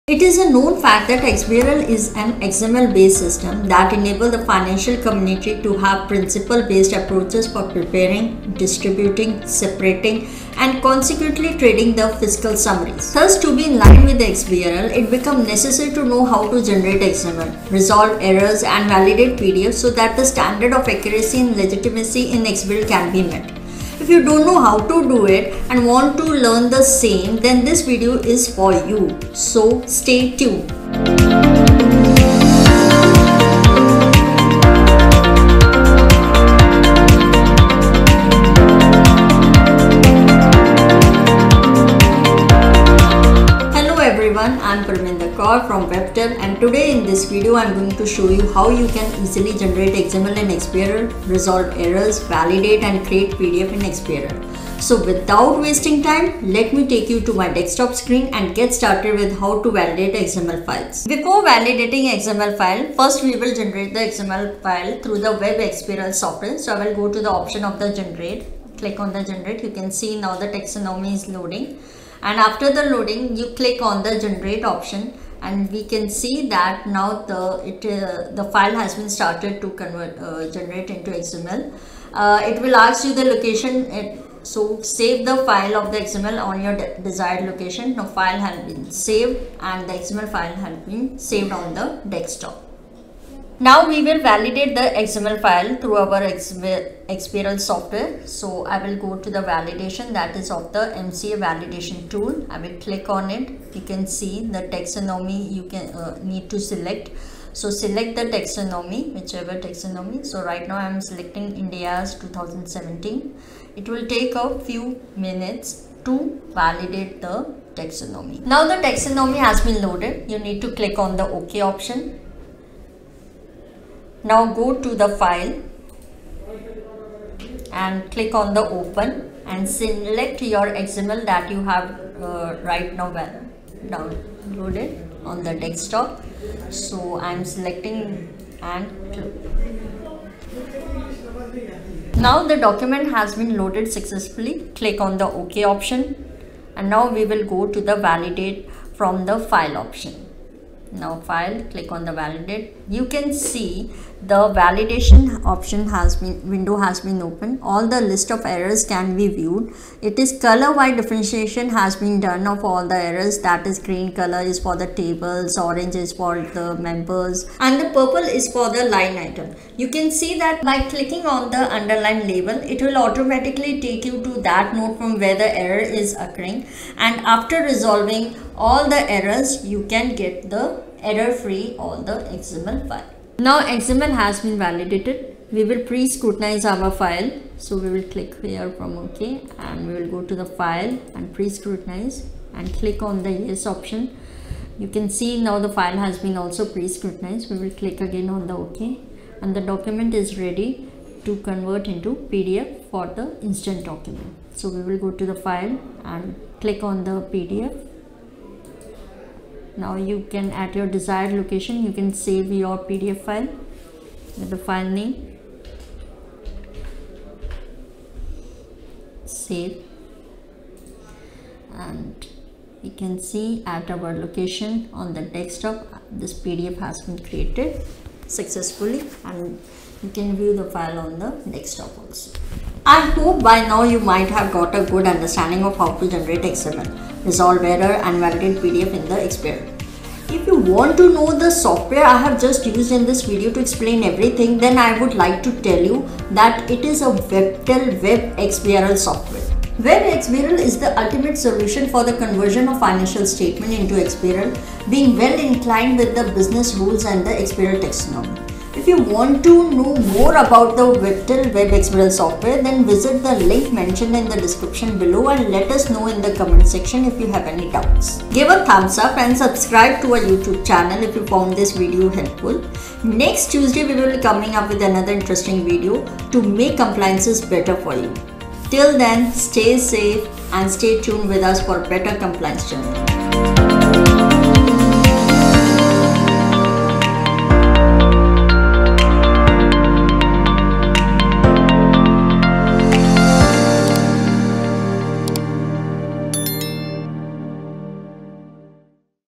How to Generate XML, Resolve Errors, Validate PDF in XBRL
Proper filing of XBRL report is a long process and generating XML, resolving errors and validating PDF are three among the most important task of XBRL filing.

In this video of Compliance Tuesday, we will tell you these three processes i.e. How to generate XML, resolve error, and validate pdf in XBRL filing.

Along with telling you about this process of XBRL filing, we will also show you the easiest way to file the XBRL report of a company.

🔹Watch this full video till the end and you will know the following:🔹
What is XBRL?
What is XML in XBRL?
How to generate XML in XBRL?
How to resolve errors while XBRL filing?
How to validate pdf in XBRL?
Which is the best XBRL filing software?